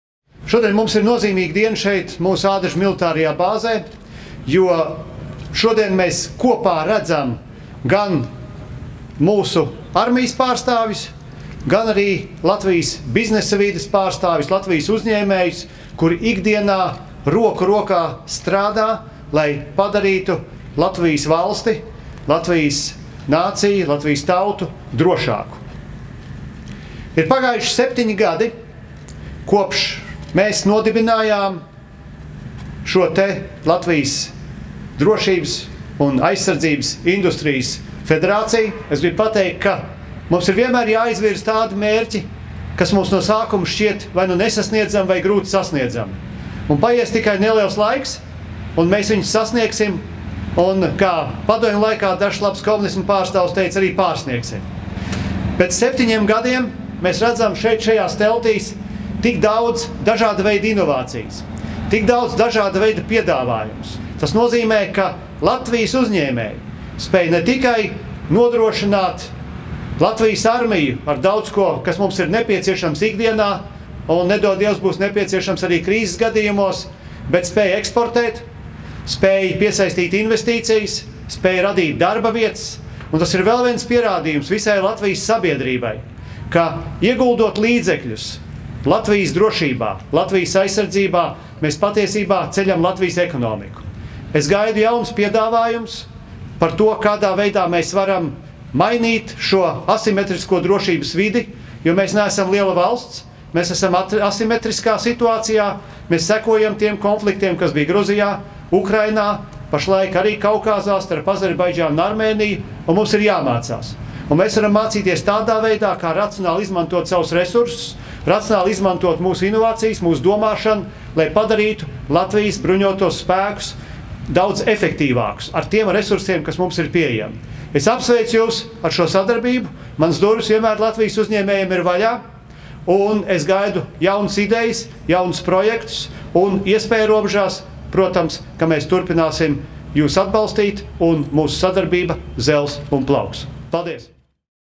Aizsardzības industrijas uzņēmumi īpašā izstādē Ādažu bāzē iepazīstina ar jauniem produktiem
Lai iepazīstinātu Nacionālo bruņoto spēku un arī Latvijā esošo NATO sabiedroto spēku pārstāvjus ar inovatīviem bruņoto spēku nodrošinājumā un apgādes sistēmā pielietojamiem produktiem un pakalpojumiem, Ādažu bāzē norisinājās Latvijas aizsardzības industrijas uzņēmumu spēju demonstrācijas izstāde.